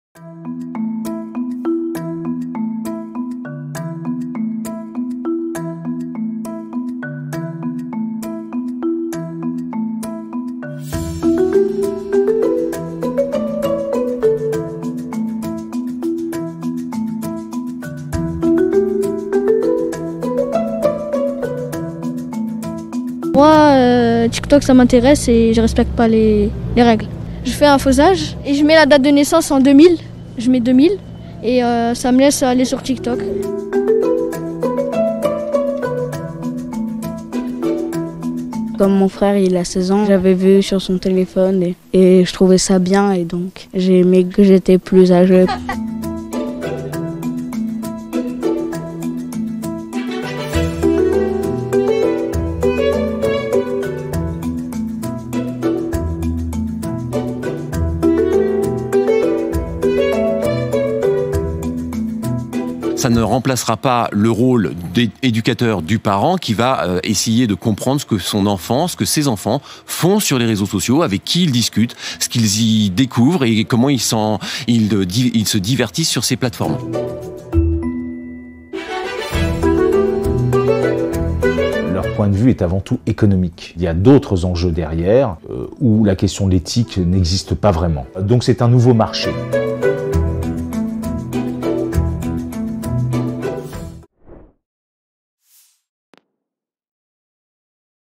Facebook veut créer "Instagram Junior", un réseau social réservé aux moins de 13 ans
Le géant Facebook planche actuellement sur le développement d'un Instagram junior.